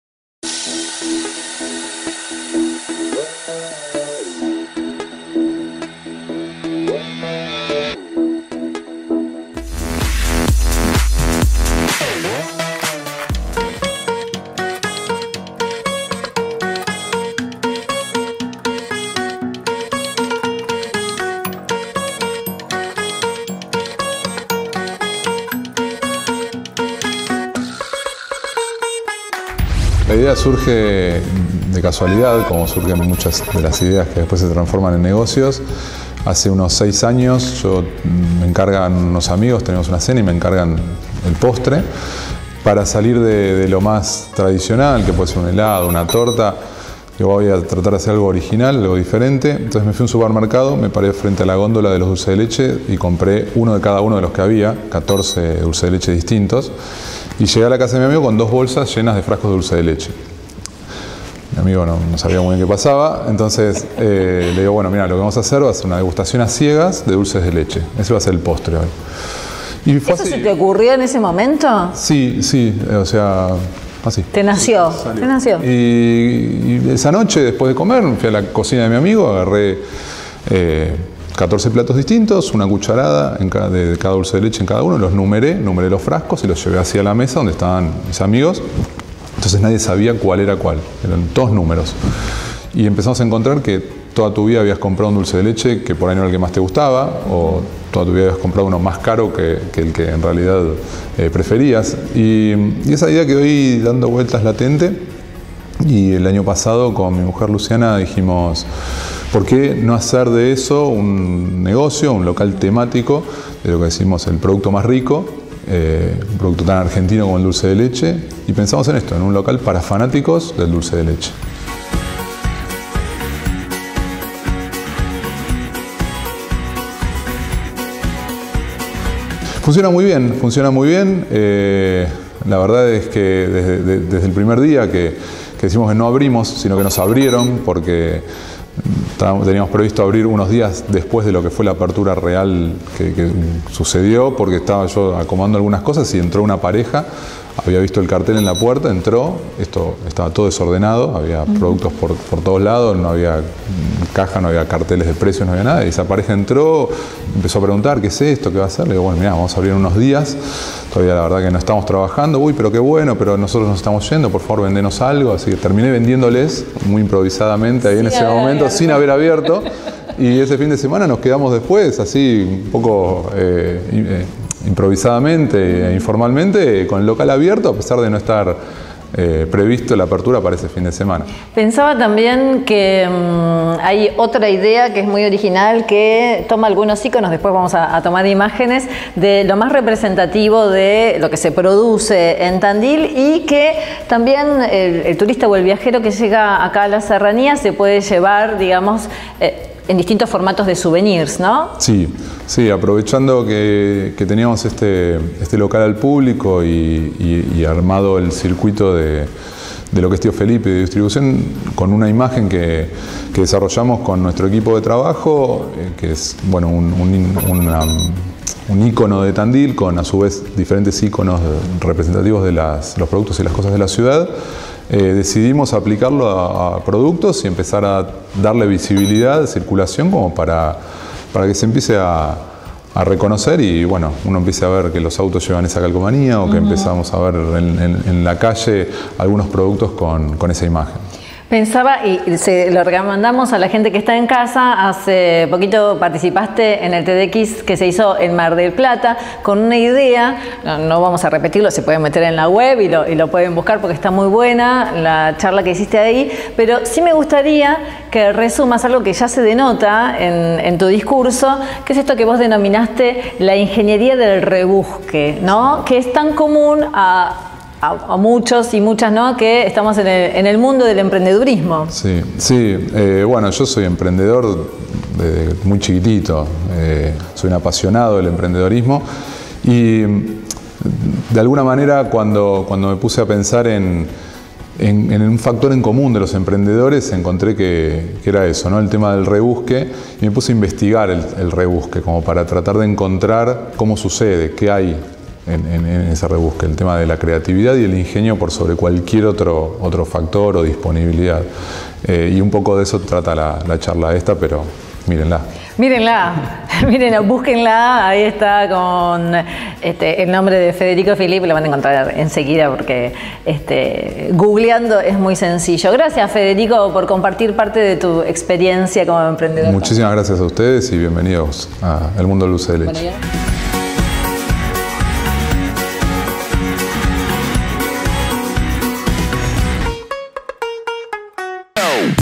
Miradas en Conjunto - El Mundo del Dulce de Leche
Con cuchara en mano nos metimos en El Mundo del Dulce de Leche: un local temático en donde confluyen todas las variedades y marcas de Dulce de Leche